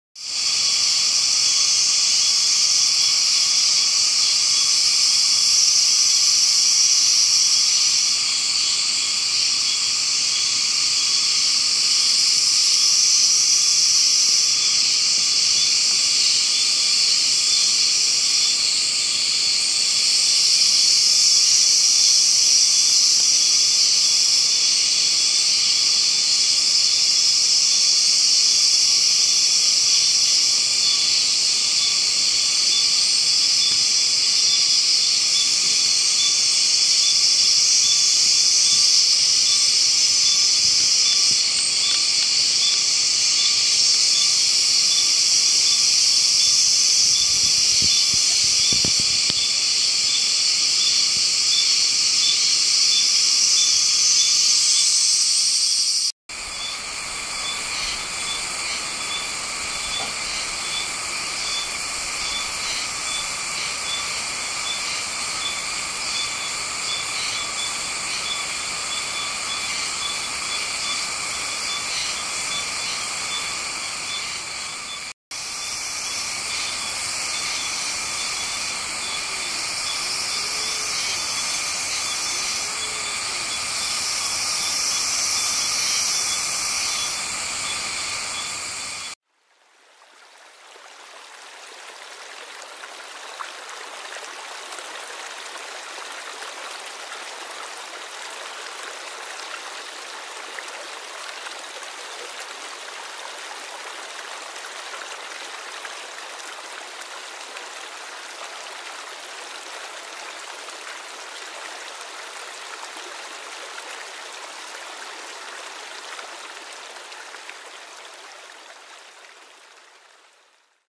アマミハナサキガエル
渓流環境。
アマミハナサキガエルの茶系と緑系の個体がいたので撮ってみました。